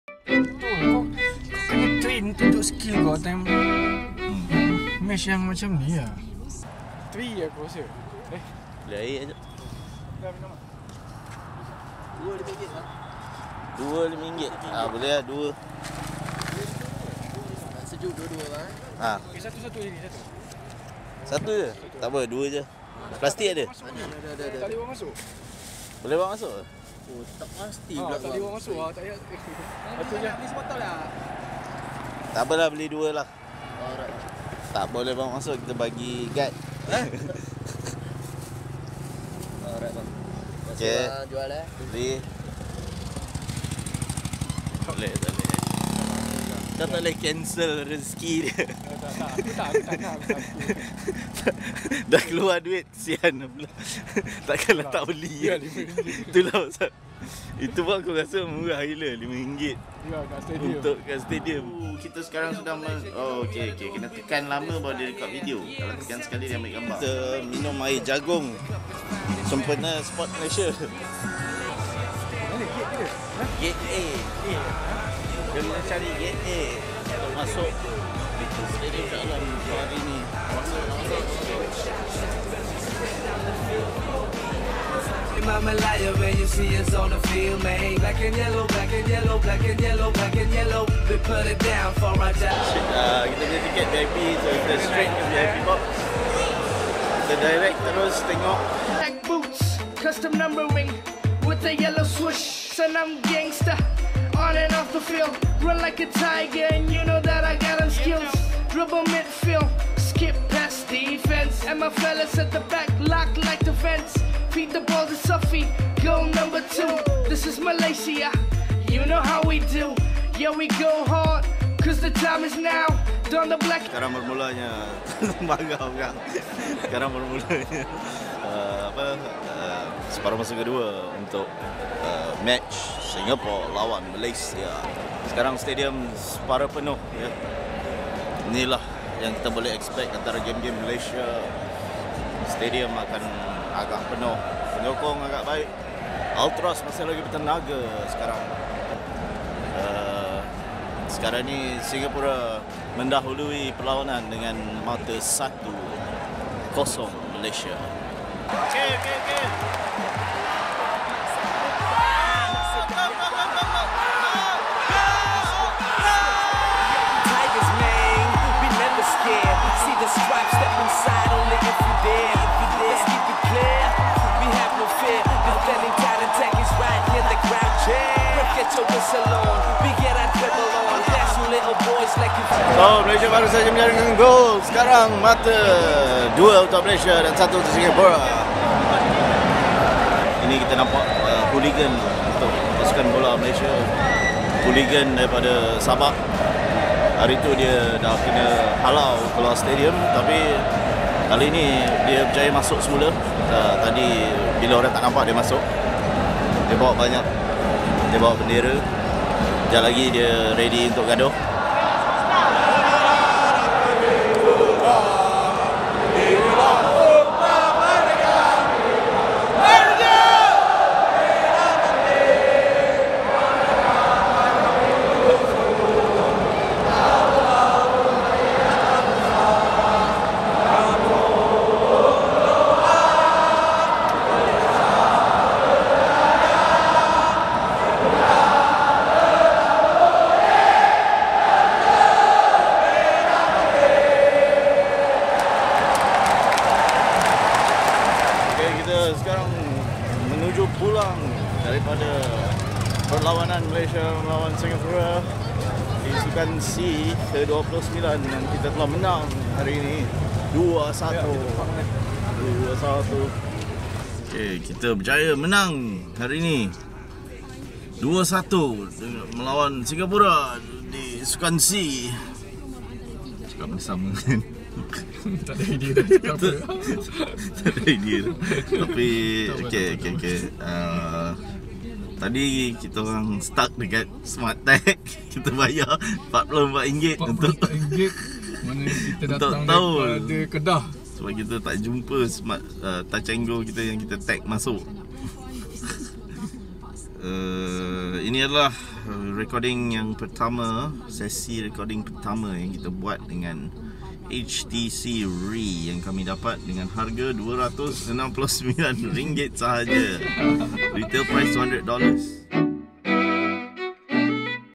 MALAYSIA VS. SINGAPURA (Sukan SEA 29, Kuala Lumpur 2017) - Recorded with HTC Re Action Camera
Suasana Perlawanan MALAYSIA V. SINGAPURA di Sukan SEA ke-29 yang lepas di Stadium Shah Alam